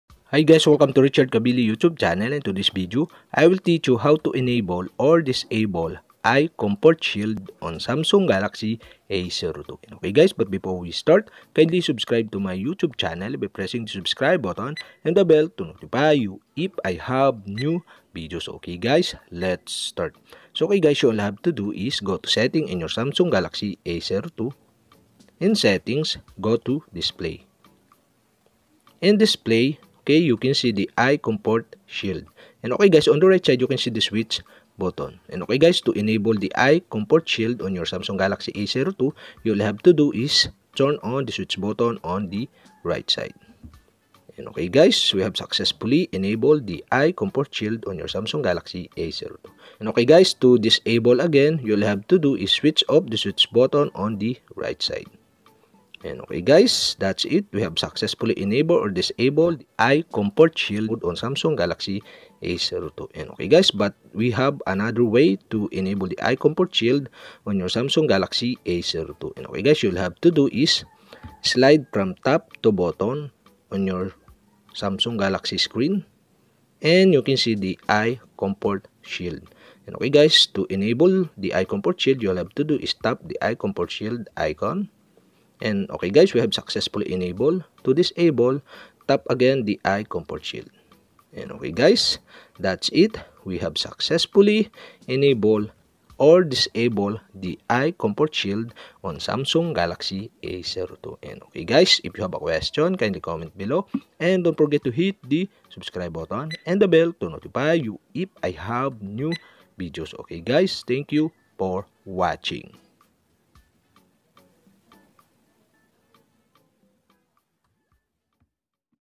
How to Enable/Disable Eye Comfort Shield on Samsung Galaxy A02
Do you want to enable or disable eye comfort shield on samsung.  In this tutorial I will teach you how to enable or disable eye comfort shield on Samsung galaxy A02 mobile phone.

Thanks...